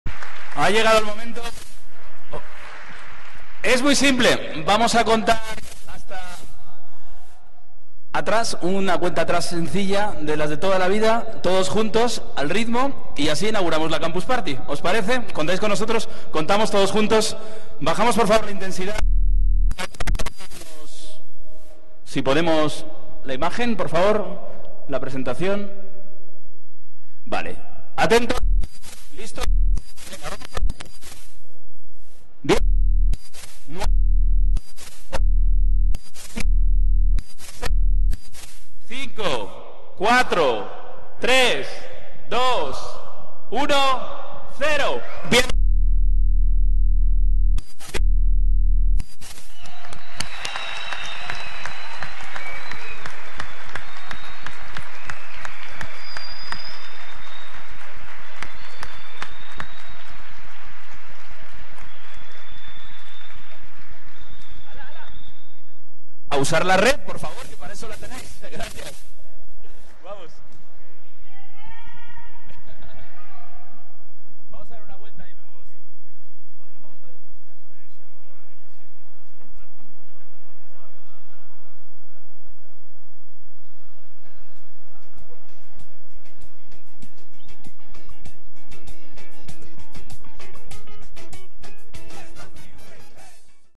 CPIberoamérica - Ceremonia inauguración (9/9)
Inauguración Campus Party Iberoamérica, conteo para darle la bienvenida a este evento.